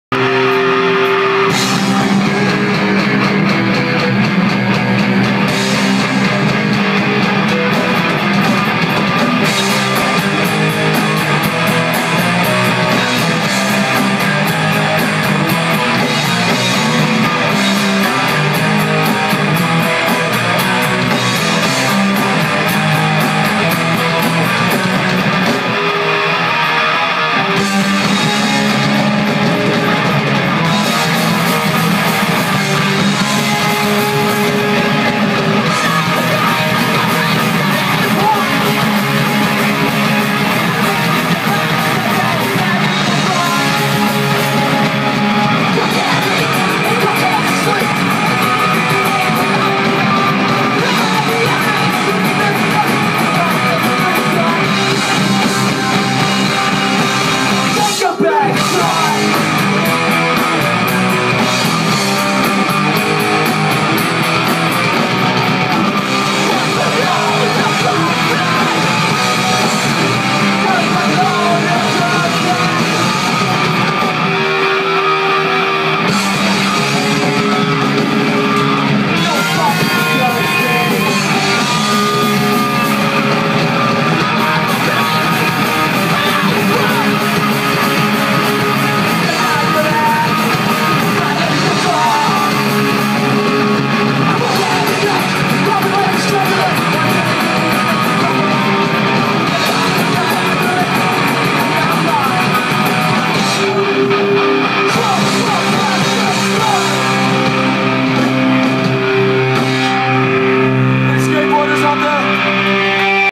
D.A. At Corktown Tavern.
Deliberate Abuse live at Corktown Tavern.